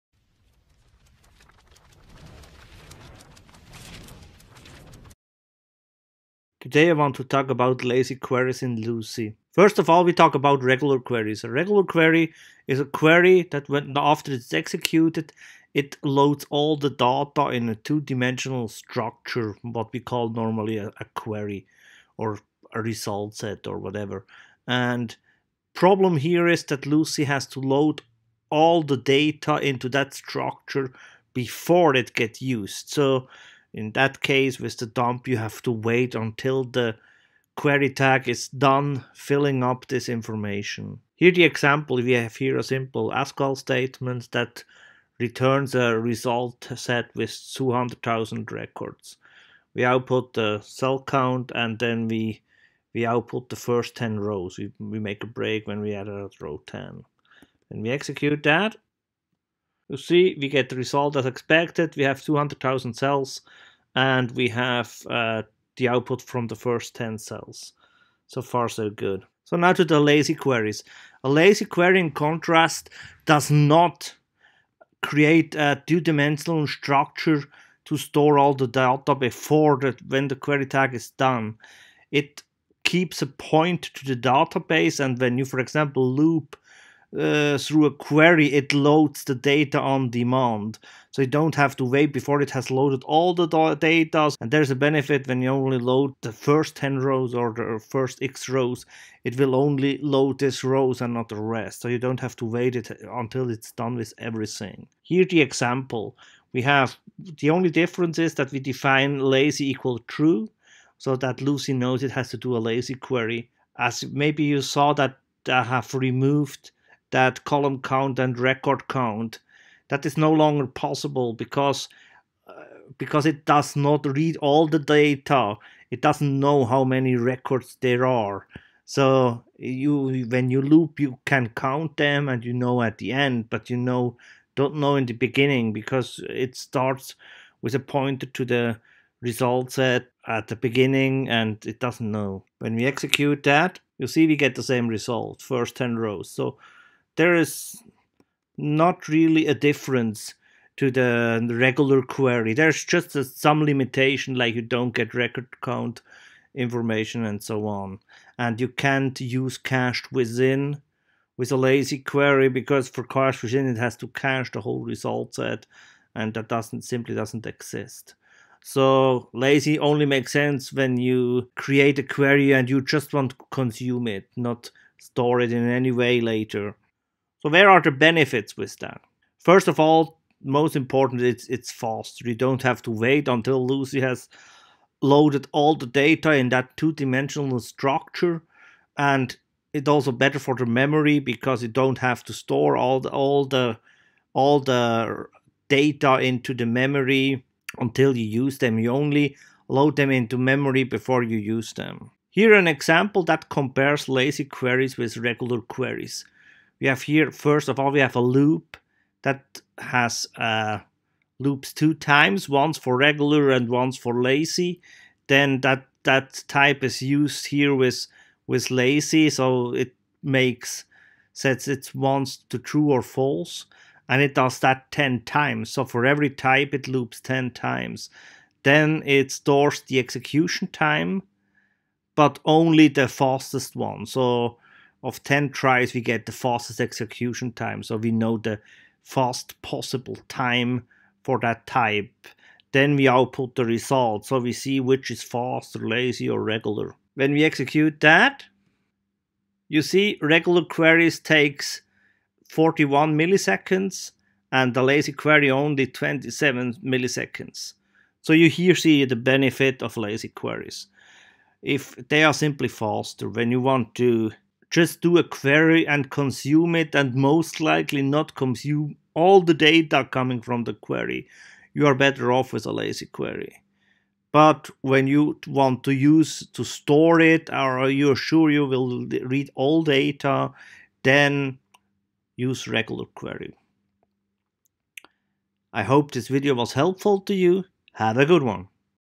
Lazy Queries
This Video explains the difference between regular and lazy queries and what they are good for.